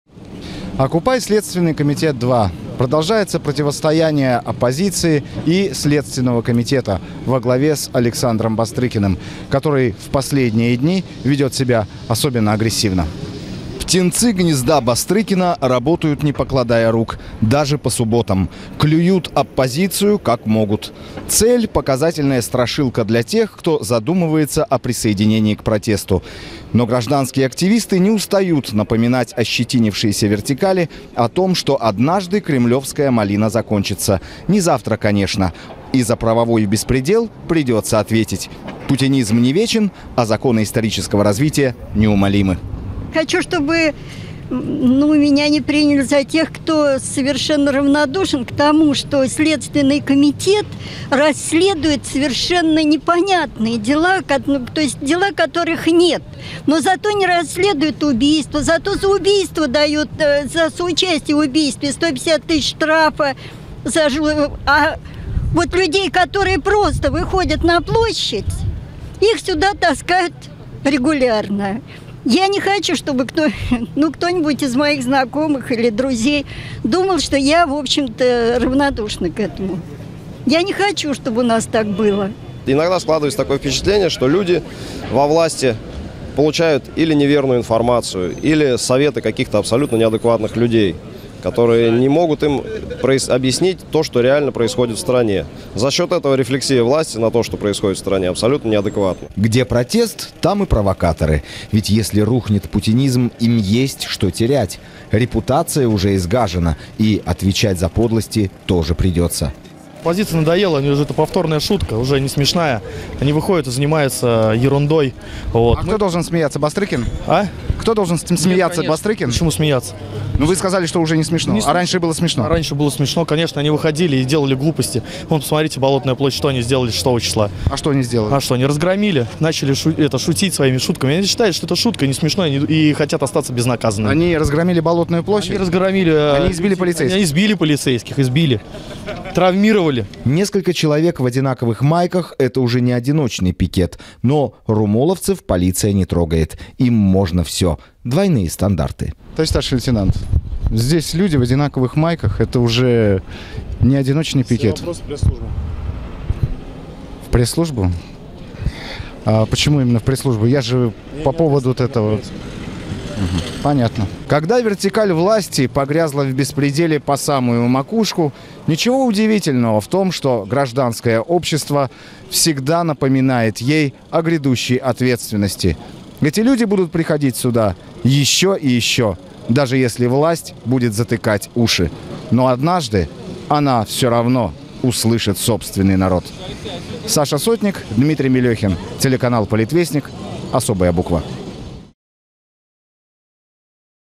Оккупай Бастрыкин.
23 июня у здания СКР состоялся многочисленный пикет в защиту политзаключённых, арестованных по делу о массовых беспорядках 6 мая 2012г. Гражданские активисты не сомневаются в том, что беспорядки были спровоцированы и организованны Кремлём.
Смотрите репортаж politvestnik.tv